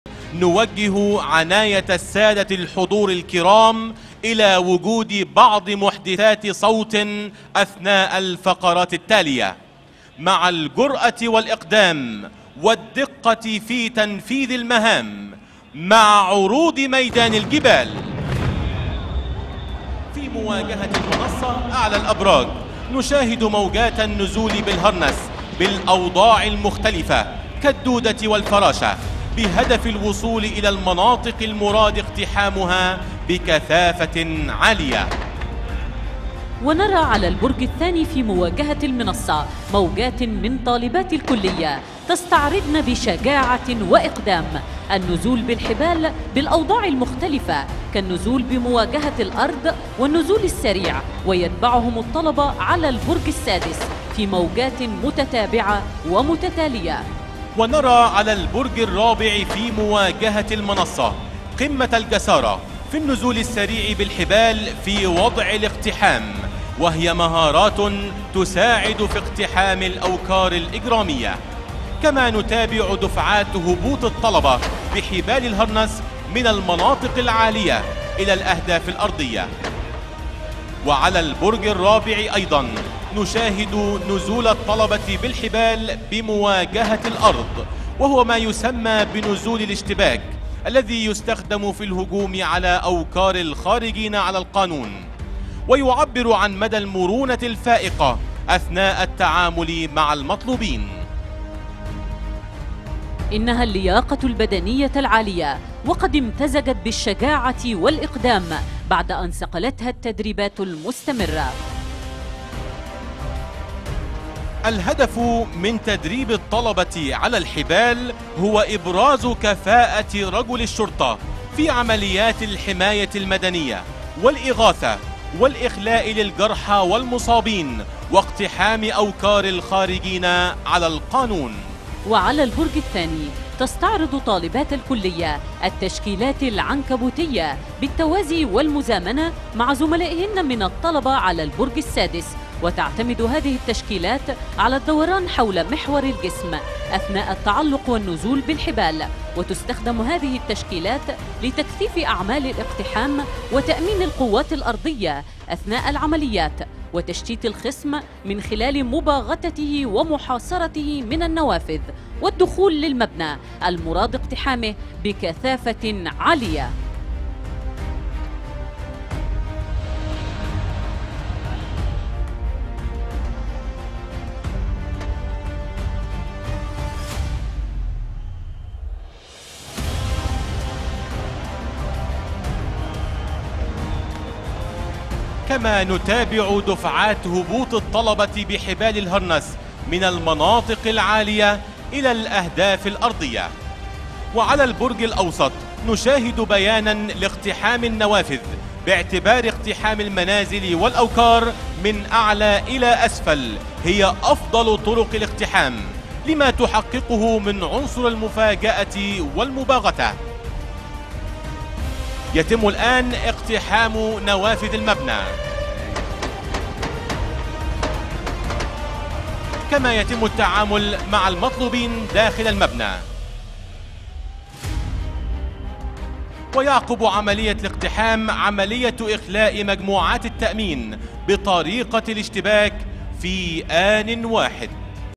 مشاهد تحبس الأنفاس لطلبة أكاديمية الشرطة خلال محاكاة عملية للاقتحامات والتعامل مع إنقاذ الرهائن
أخبار_ten - مشاهد تحبس الأنفاس لطلبة أكاديمية الشرطة خلال محاكاة عملية للاقتحامات والتعامل مع إنقاذ الرهائن